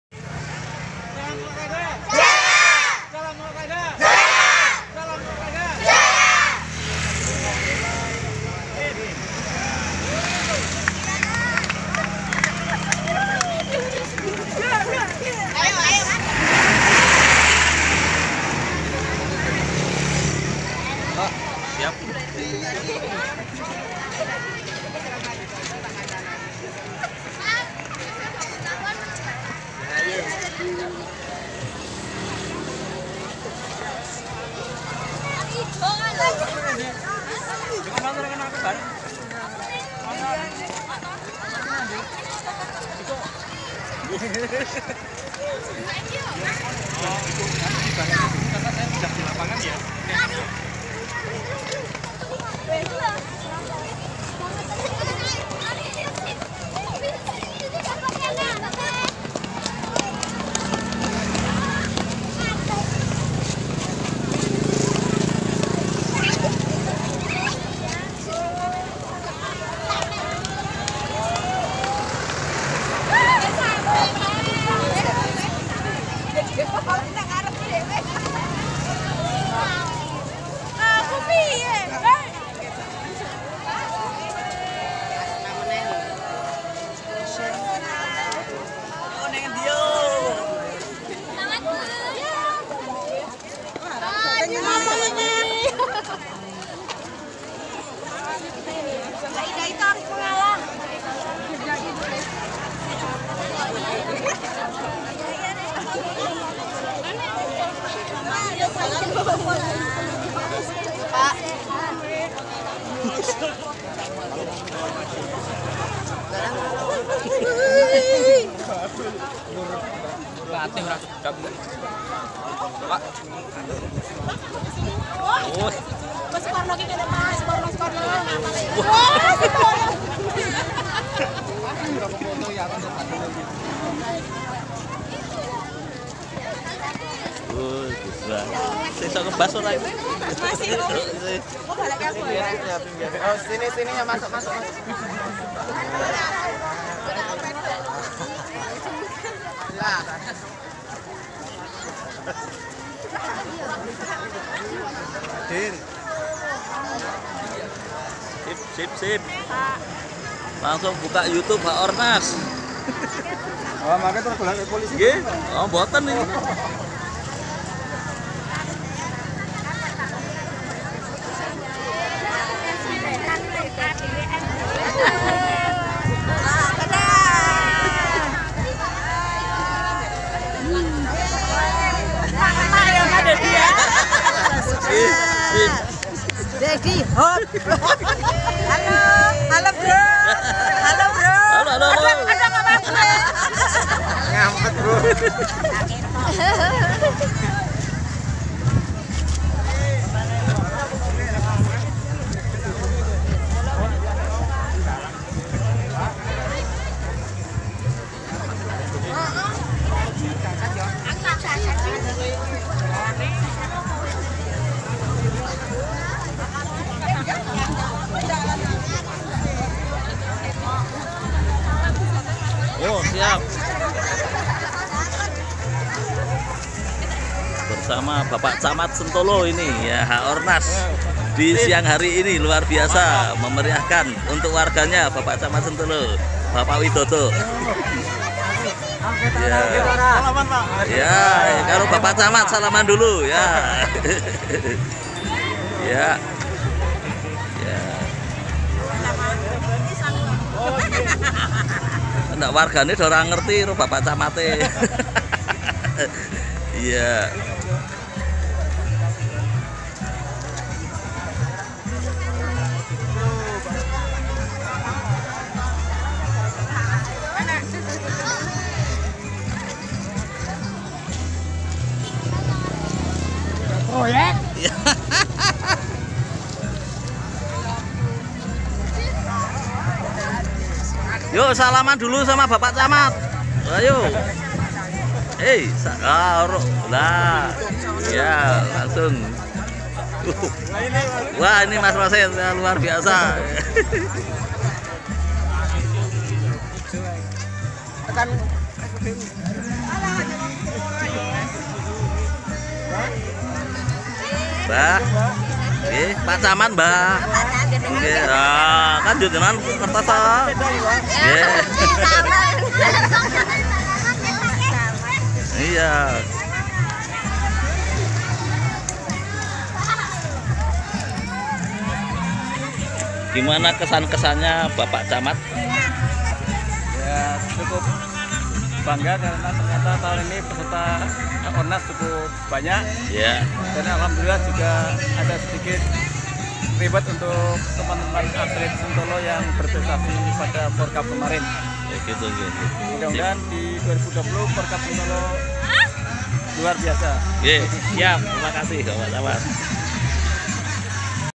HAORNAS 2019 KECAMATAN Sentolo, ft P Widodo/Camat Sentolo dg JALAN SEHAT di kaliagung Jogjakarta
Dengan semangat HAORNAS di Kecamatan Sentolo bersama bp Widodo/Camat sentolo beserta seluruh warga,siswa siswi dari sd,smp,sma,berbagai instansi di wilayah kecamatan sentolo dan sekitarnya
Diadakan kegiatan tahunan yaitu Jalan sehat sejauh 2km di desa kali agung, berbagi senam berdurasi 30 menit, membuat antosias warga tergugah karena merasa segar kembali yang di rasakan,apalagi jalan sehat sejauh 2km cukup membuat tubuh membara rasanya, karena seluruh otot otot bergerak dan beraktivitas sehingga lemak lemak yang mengendap di dalam tubuh kita bisa terbuang, dan juga dengan adanya senam yang berdurasi 30m cukup sekali mengeluarkan keringat yang sehat
Bersama bp Camat sentolo bp Widodo
Dengan semangat memekikkan SALAM OLAH RAGA sambil mengangkat genggaman tangan kanan nya.

olah raga